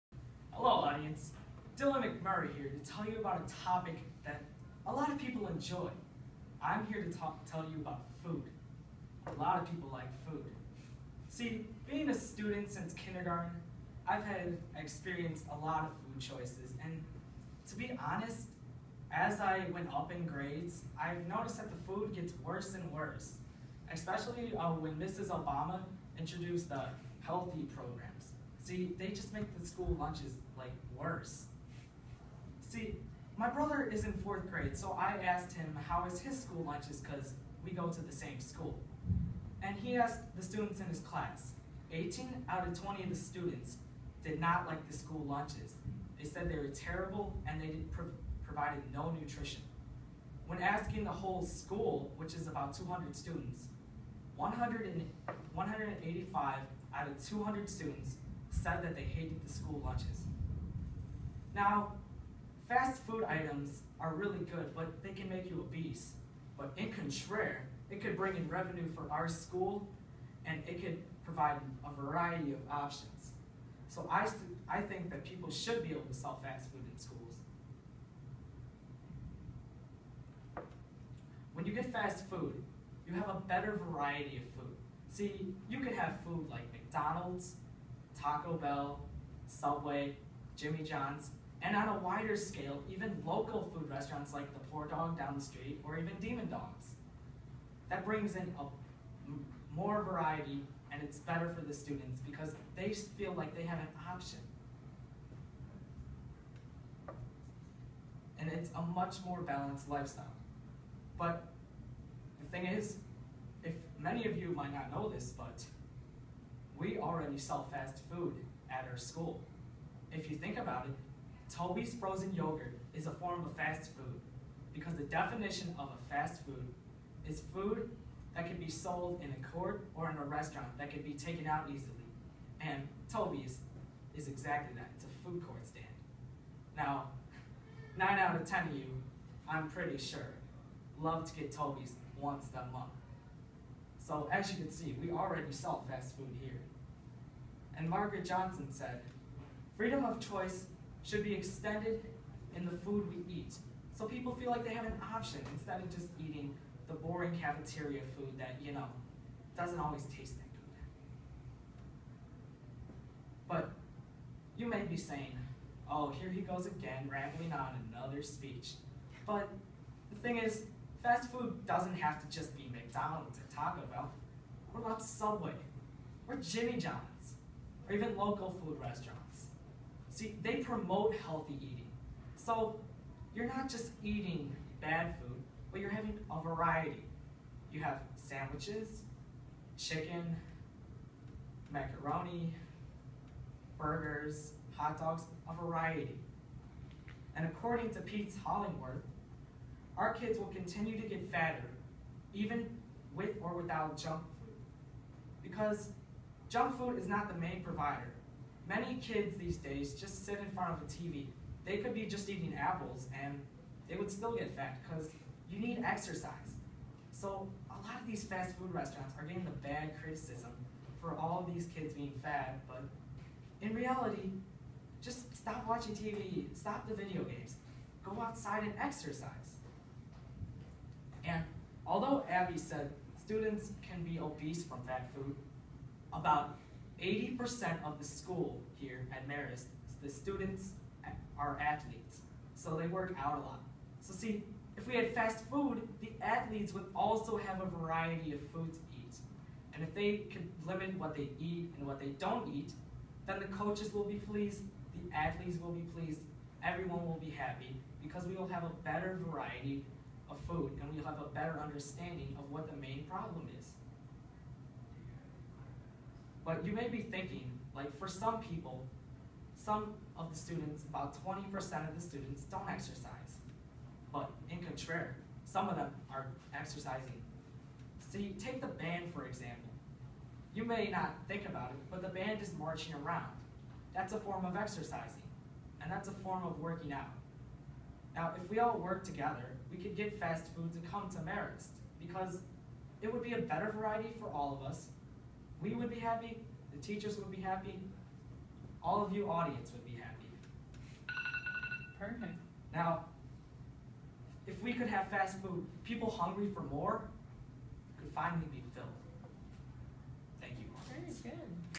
Should Schools Offer Fast Food Options? (For)
NOTE: (This Speech was produced and recorded on November 25, 2014; but I waited to upload it today on December 1, 2014 because it is a new month and that is what this video needs a fresh start because it is terrible!) I thought my Sales Pitch Speech was bad! This Speech is by far my worst! This Speech features: biased arguments, straying from the main point, missing the main arguments, of course; SWAYING, and the worst thing; an audience. I just presented this in front of my great teacher Mrs. Paoletti and a random senior girl that recorded me, shoutouts! This Speech WILL be the one chosen to be redone for my Final Speech as I look extremelly forward to it!